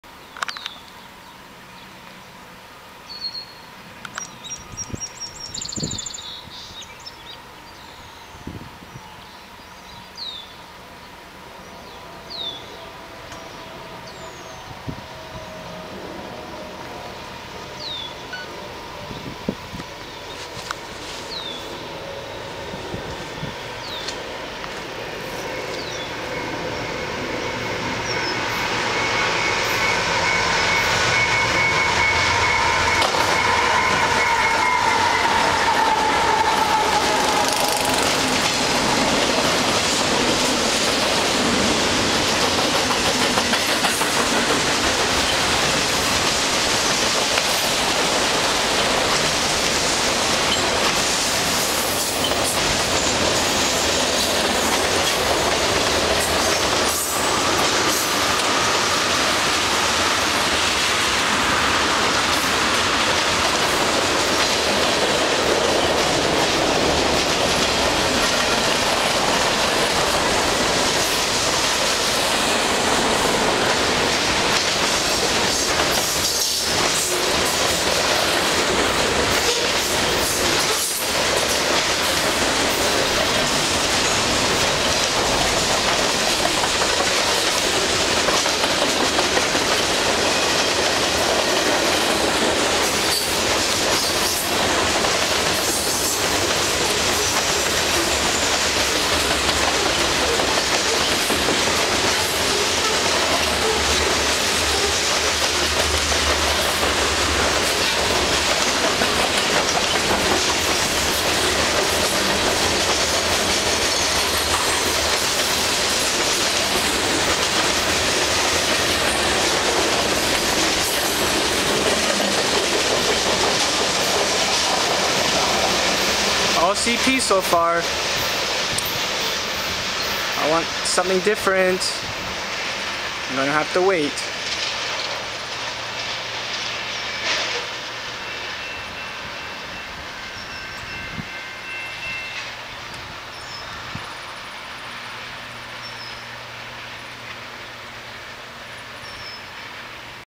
CP Engines 8803, 8510 & 5872 lead this long EB Intermodal/Autoracker/mixed freight thru Lisgar GO
heres my 5th catch from today. Luke (VIA909) had caught this one this morning where he lives. never knew it was headed my way haha. this train came 4mins after the WB CP 5 engine lash-up.

video taken: Monday May 24th 2010 at 12:18pm
and as always, Comment & Enjoy !
thnxs ! =]